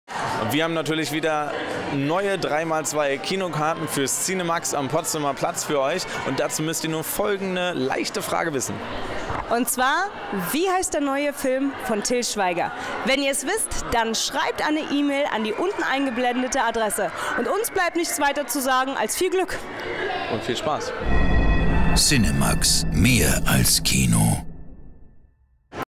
Cinemaxx Gewinnspiel KW 4
Du magst Filme in Spitzenqualität und am Besten kostenlos? Dann bist Du bei uns genau richtig, wir verlosen gemeinsam mit unserem Partner dem Cinemaxx am Potsdamer Platz wöchentlich 3×2 Kinogutscheine. Frage beantworten, an uns schicken und gewinnen!